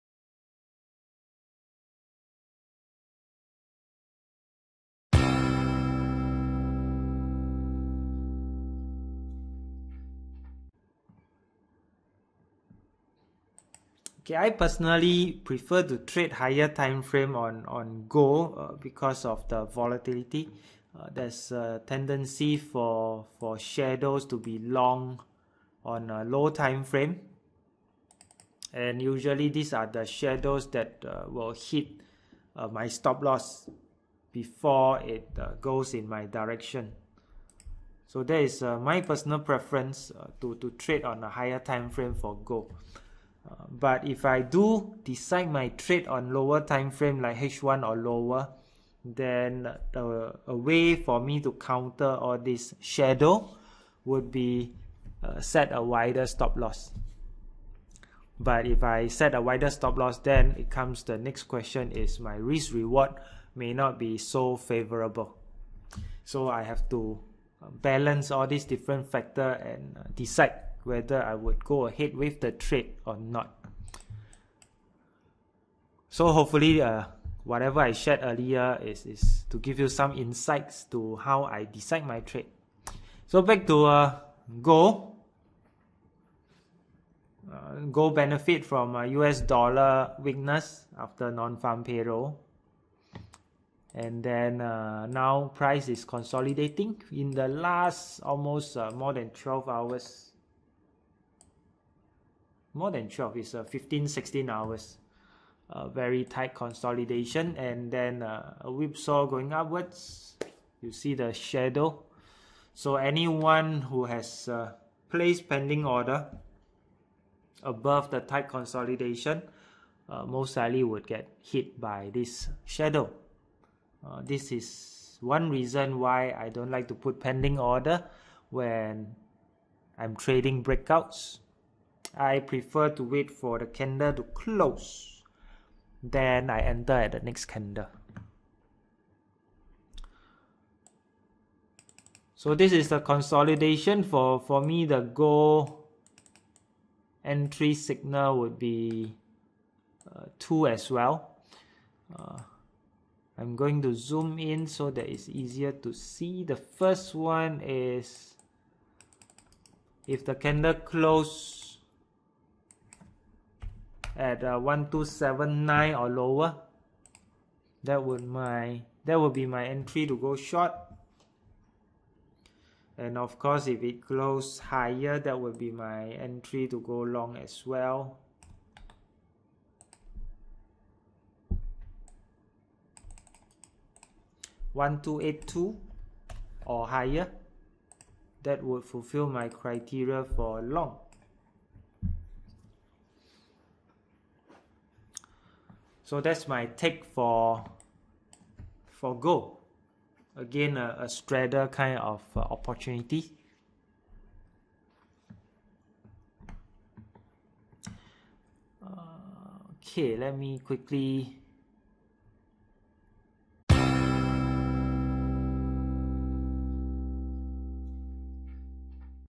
The Gold Straddle
Head of Research & Education Fullerton Markets Ltd Wayne Ko offers his weekly webinar on what’s going on in the currency markets and what will happen in the next days. Ko also analyzes the market on real time.

The webinar brings three segments:

Review of the events of the last week
Preview of the most important data for the week
Charts and trading opportunities.